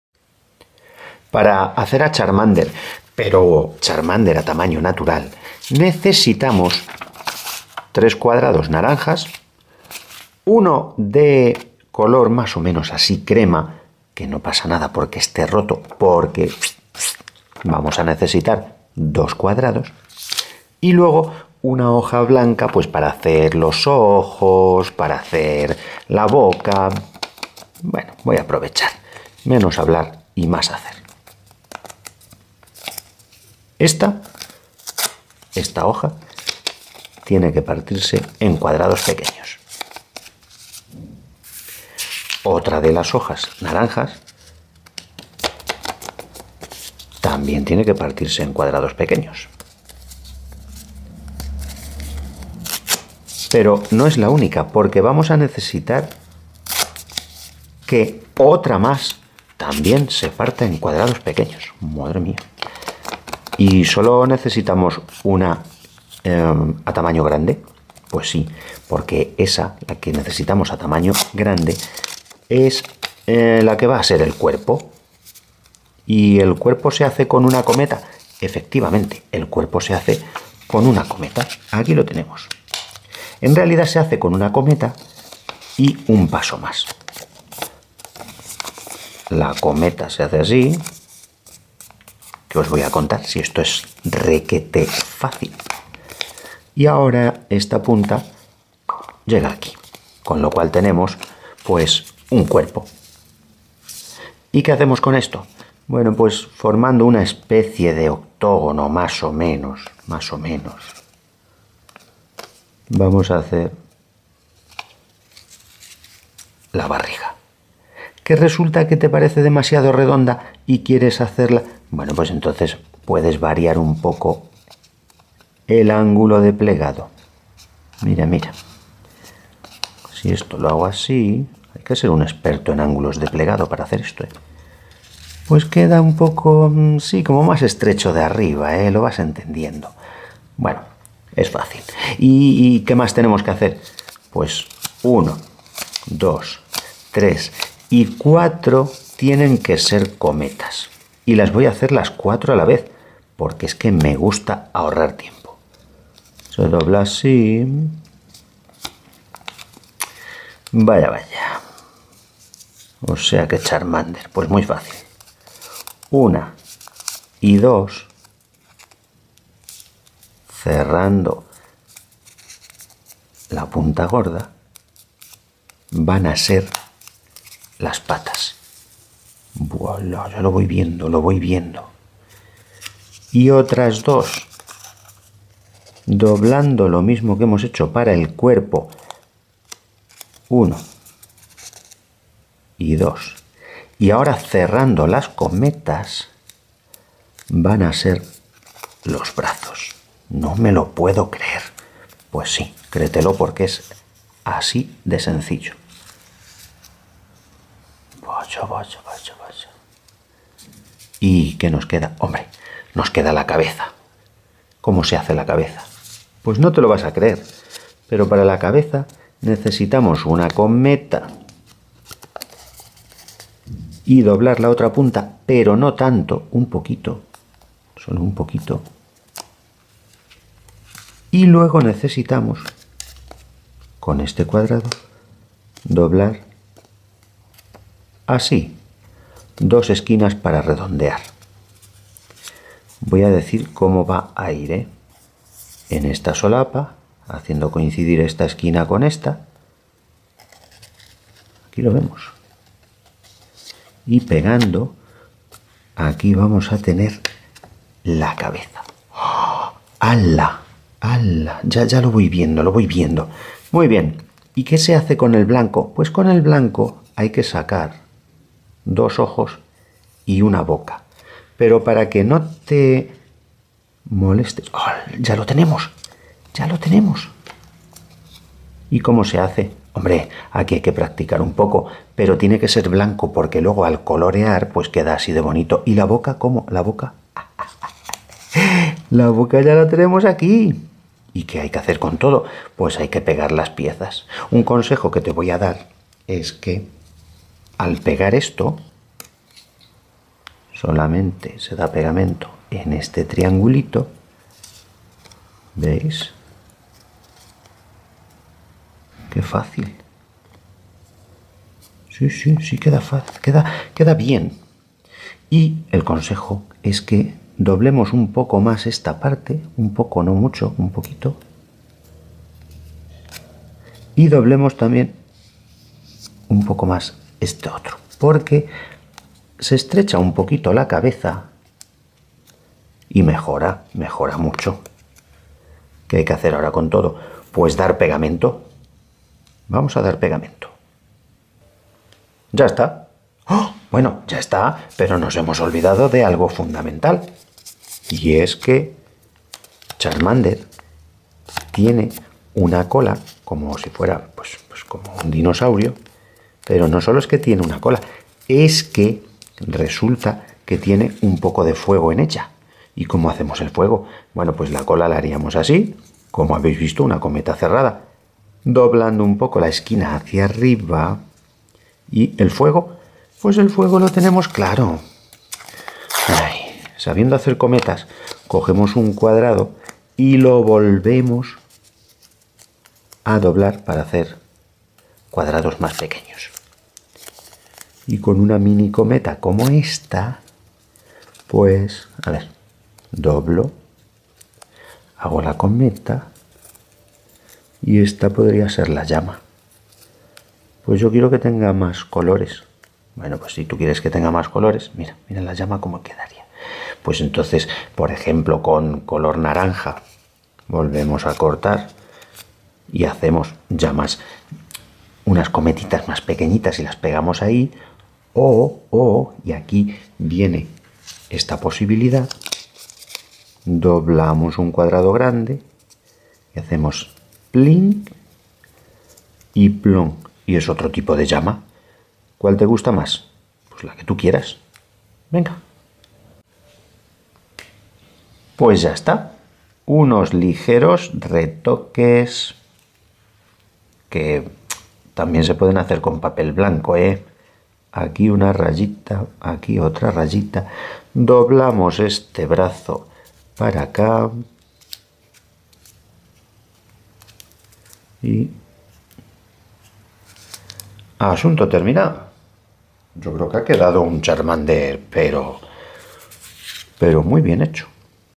Pokemon. Charmander
Charmander de papel. Se pliega con cuatro cuadrados y el sistema C de papiroflexia a base de cometas.